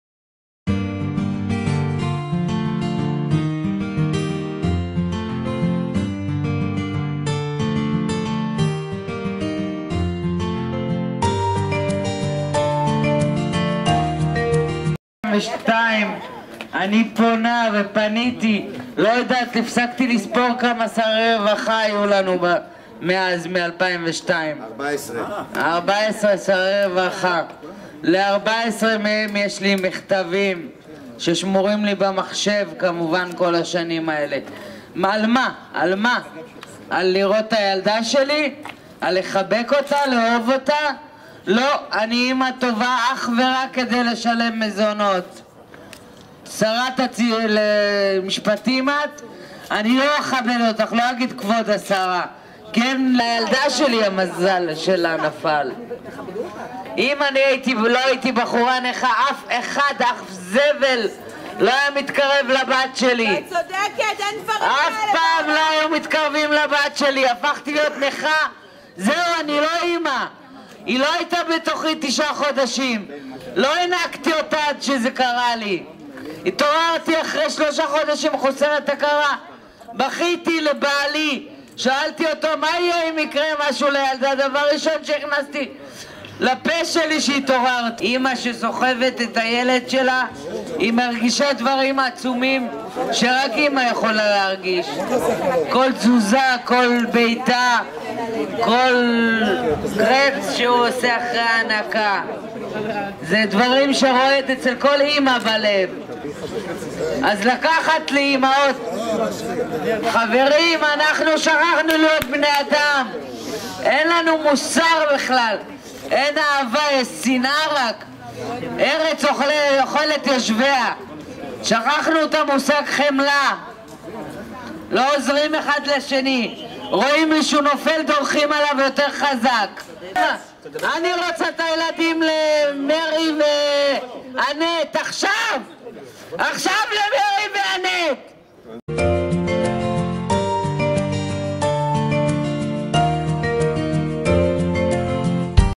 דנית לוין אמא שמשרד הרווחה חטף את בתה לפני 12 שנים - הפגנה מול בית שרת המשפטים
דנית לוין, אמא שמשרד הרווחה חטף את בתה לפני 12 שנים
זועקת את זעקת האמהות שילדיהן נחטפו מהן בעוולה חמורה.
22 במאי 2014
הפגנה מול הבית של שרת המשפטים ציפי לבני
פניתי משנת 2002 ועד היום ל- 14 שרי רווחה שהתחלפו - שום עזרה לא קיבלתי. 12 שנים אני מנותקת מהבת שלי. אם לא הייתי נכה, אף אחד לא היה יכול לקחת לי את הילדה שלי.
דנית לוין, אמא.